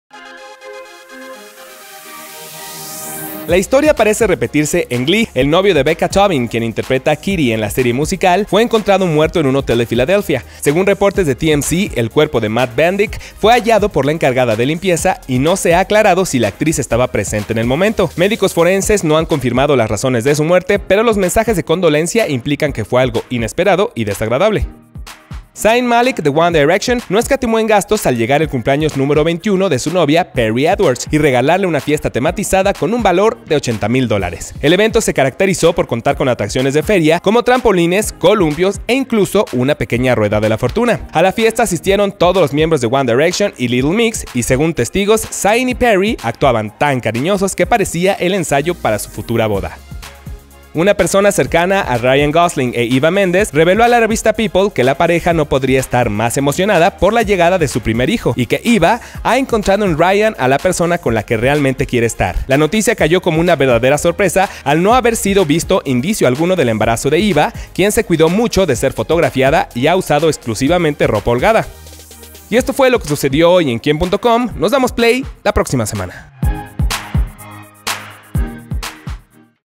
Muere novio de otra chica Glee; Zayn hace super fiesta
El novio de Becca Tobin, actriz de Glee, fue encontrado muerto en un hotel; Zayn Malik hizo increíble fiesta con juegos de feria para su novia Perrie; Ryan y Eva están muy emocionados por su bebé.